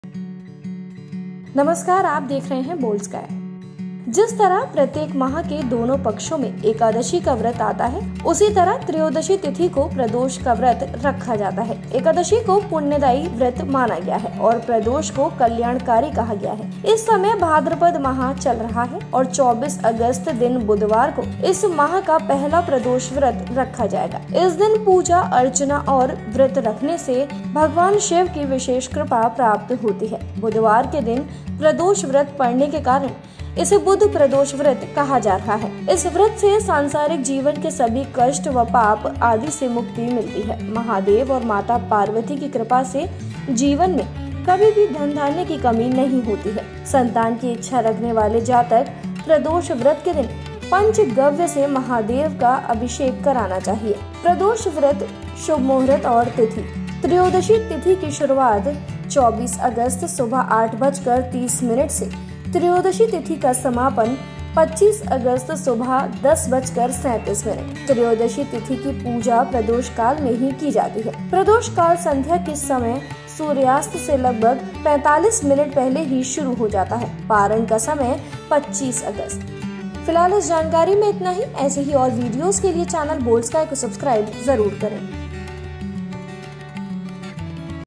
Budh Pradosh Vrat 2022: बुध प्रदोष व्रत कब है 2022 | Budh Pradosh Vrat Shubh Muhurat 2022 *Religious
प्रदोष व्रत के दिन भगवान शिव के साथ माता पार्वती की भी पूजा की जाती है। त्रयोदशी तिथि का व्रत सायंकाल के समय किया जाता है इसलिए इसे प्रदोष व्रत कहा जाता है।

Goddess Parvati is also worshiped along with Lord Shiva on the day of Pradosh Vrat. The fast of Trayodashi Tithi is observed in the evening, hence it is called Pradosh Vrat.